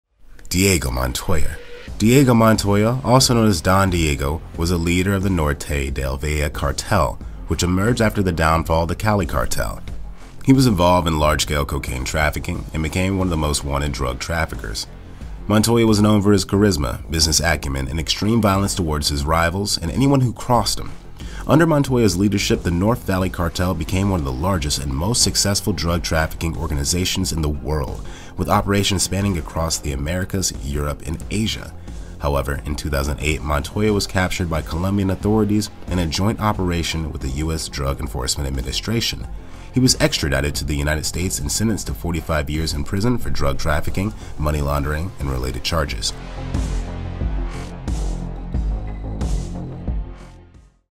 The Dark World of Diego Montoya and His Cartel Empire
Diego Montoya, also known as "Don Diego," was a leader of the Norte del Valle Cartel, which emerged after the downfall of the Cali Cartel. He was involved in large-scale cocaine trafficking and became one of the most wanted drug traffickers.

Montoya was known for his charisma, business acumen, and extreme violence towards his rivals and anyone who crossed him. Under Montoya's leadership, the North Valley Cartel became one of the largest and most successful drug trafficking organizations in the world, with operations spanning across the Americas, Europe, and Asia.

However, in 2008, Montoya was captured by Colombian authorities in a joint operation with the US Drug Enforcement Administration (DEA). He was extradited to the United States and sentenced to 45 years in prison for drug trafficking, money laundering, and related charges.